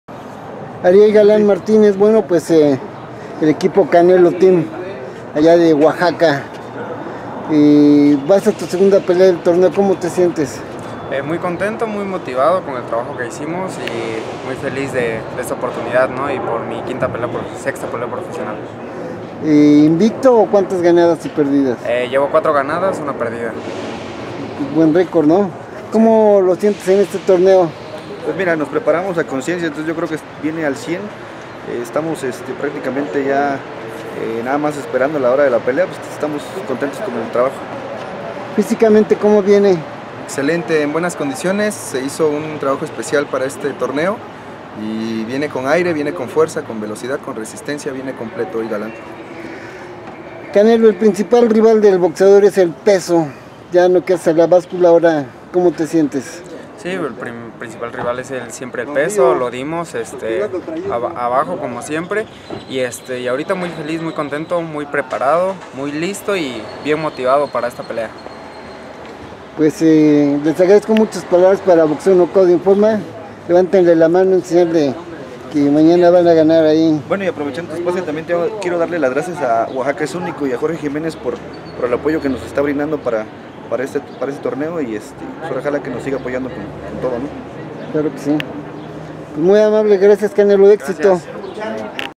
Ariel Galán "El Canelo" Martínez quiere el triunfo en Puños de Oro
El peleador oaxaqueño viene por su su segunda victoria de la justa  y aseguró que se preparo al 100 para este torneo.